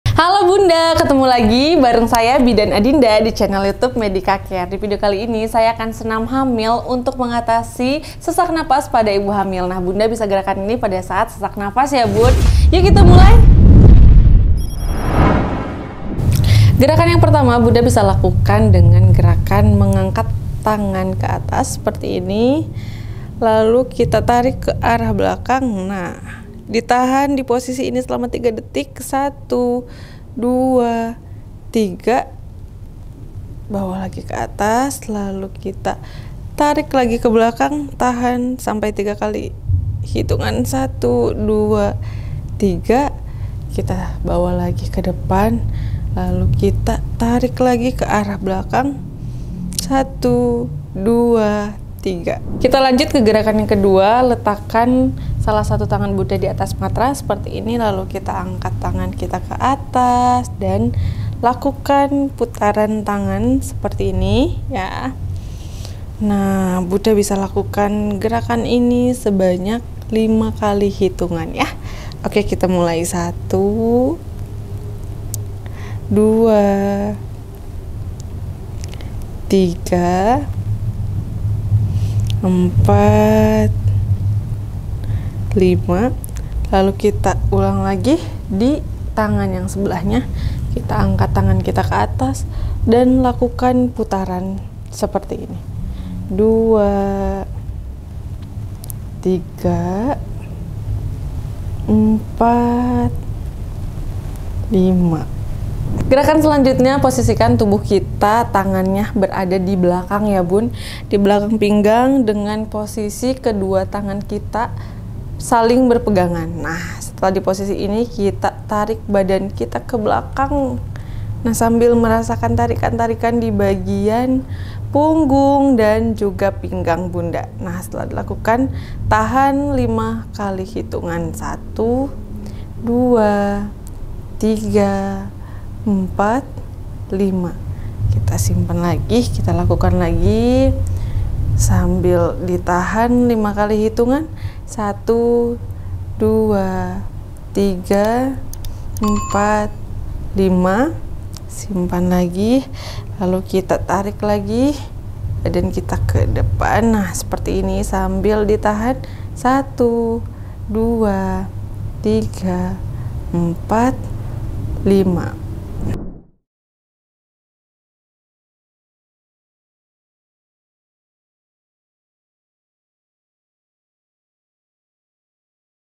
Senam Hamil mengatasi Sesak Nafas || Bidan Adinda, Amd, Keb - Medikacare
Senam hamil merupakan terapi latihan gerak dan salah satu kegiatan dalam pelayanan selama kehamilan atau prenatal care yang bertujuan untuk mempersiapkan ibu hamil secara fisik dan mental saat menghadapi persalinan agar persalinan normal dapat berlangsung dengan cepat, aman, dan spontan